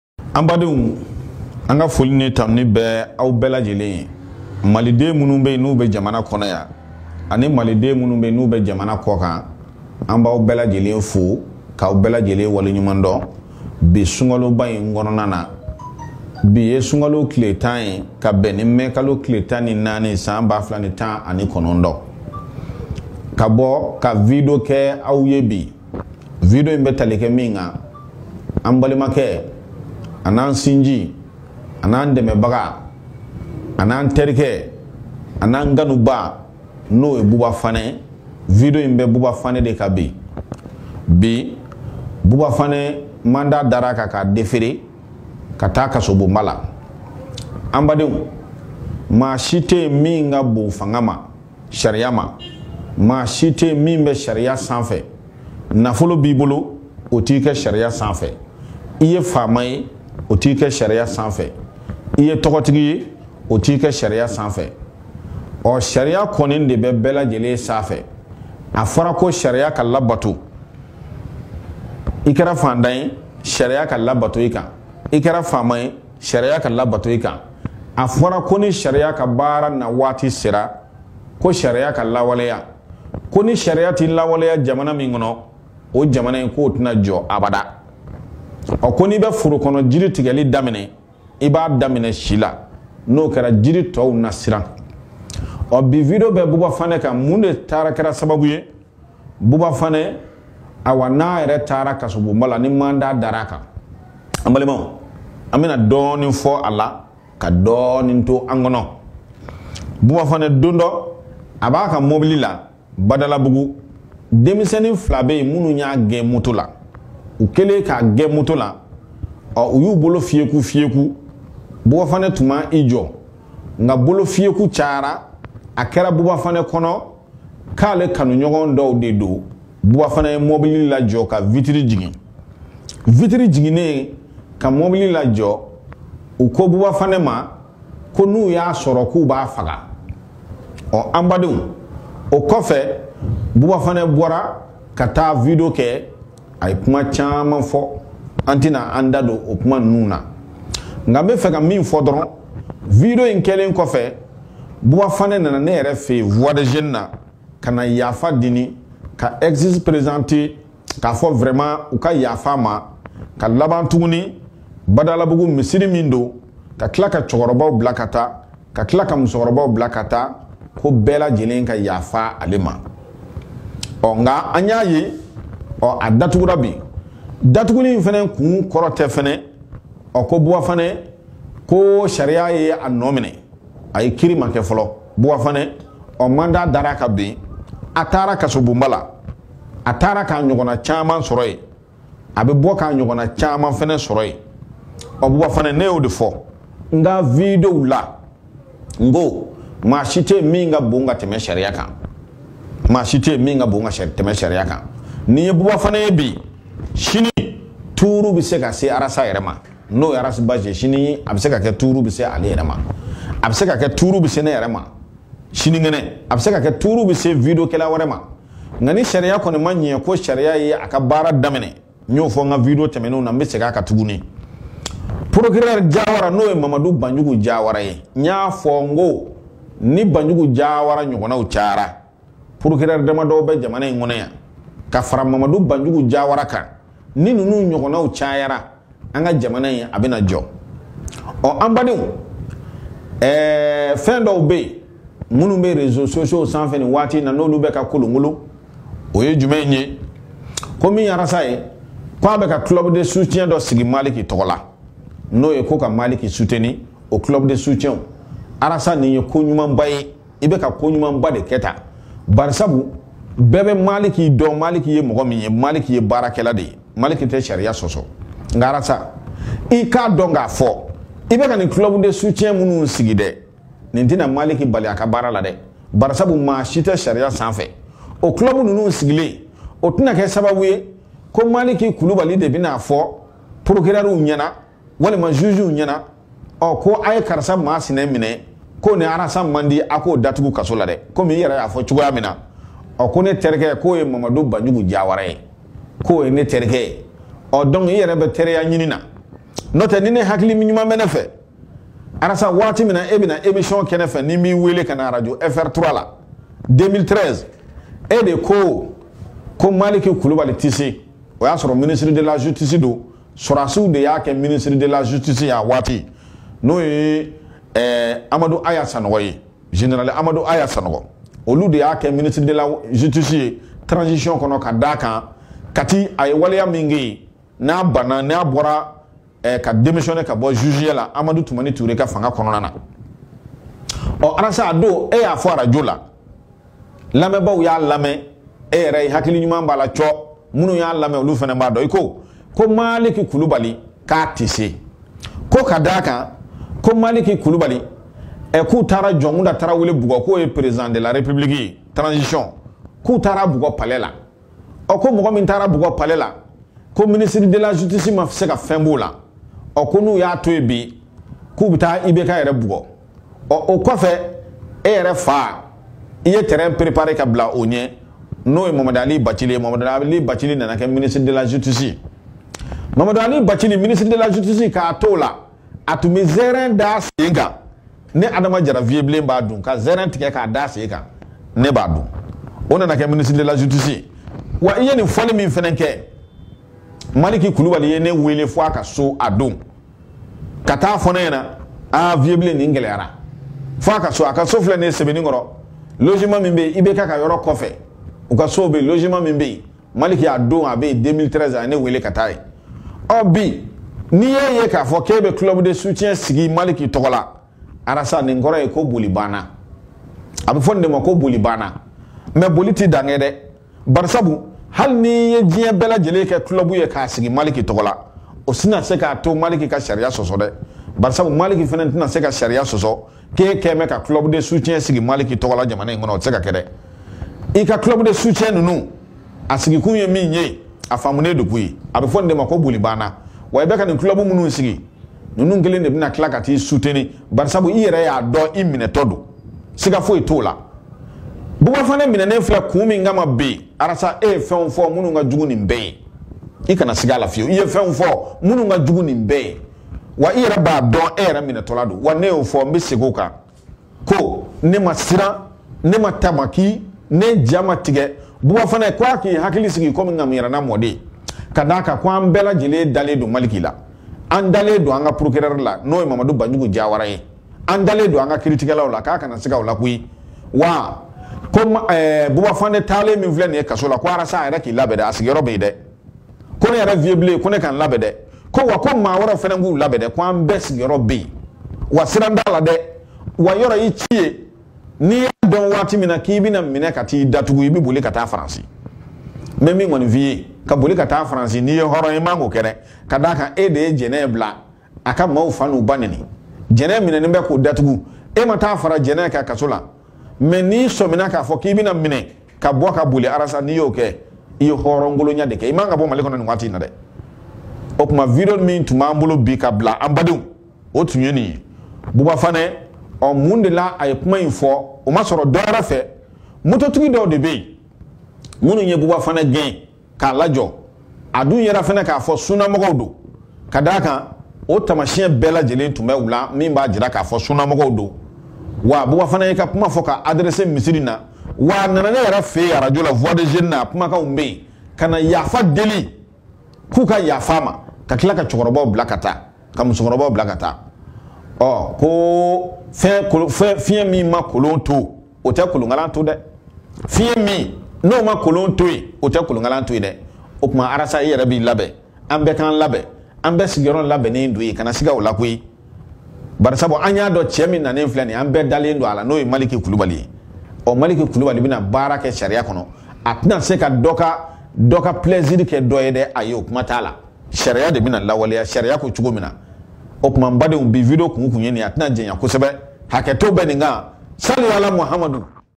URGENT-Vieux Blen Se Prononce sur L'ARRESTATION DE BOUBA FANE et Sur Le Problème Ras Bath et Malick
L'arrestation de BOUBA FANE.
Vieux Blen se prononce et revient sur le problème Malick Coulibaly , Ras Bath en 2013. Justice pour tous. À écouter sa modération. Affaire à suivre